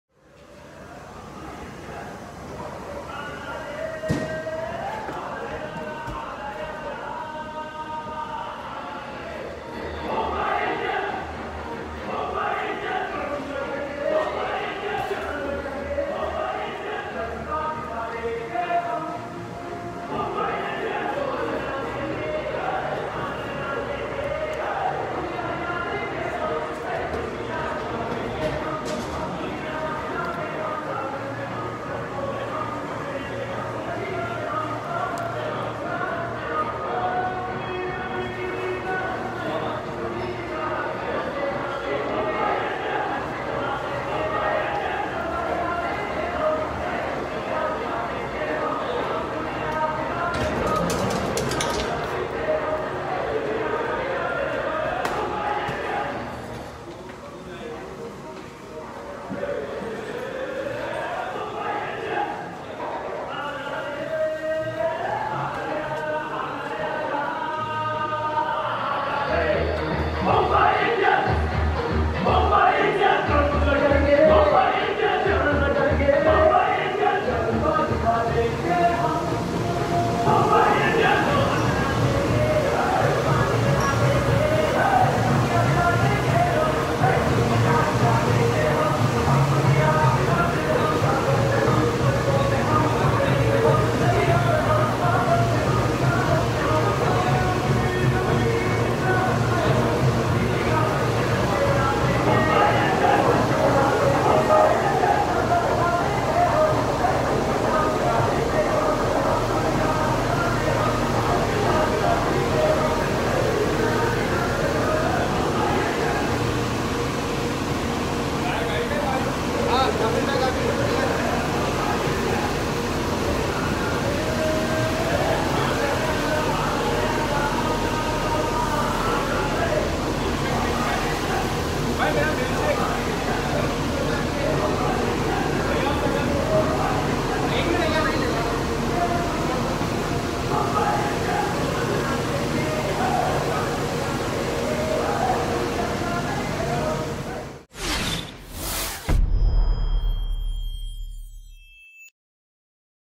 We depart for KXIP vs MI | पंजाब मैच के लिए निकली टीम | Dream11 IPL 2020
Watch the Mumbai Indians (MI) team depart for their match against Kings XI Punjab (KXIP) at Abu Dhabi, UAE in the Dream11 IPL 2020.